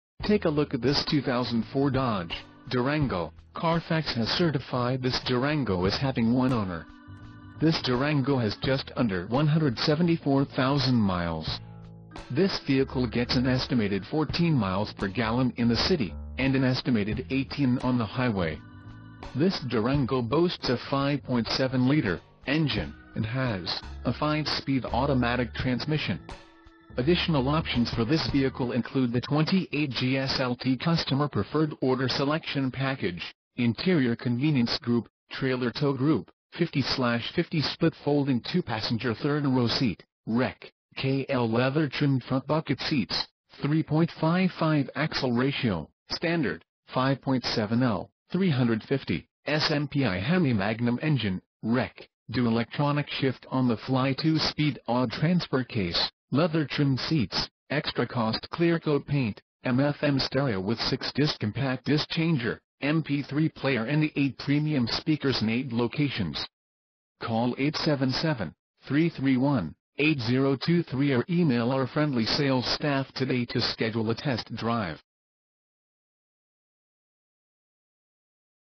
2004 Dodge Durango 4dr 4WD SLT SUV - Prince George, VA
Crossroads Chrysler Jeep Dodge, Prince George, VA - SLT 4x4 5.7L Hemi Leather 3rd Row Seat Alloy Wheels Premium Sound 1-Owner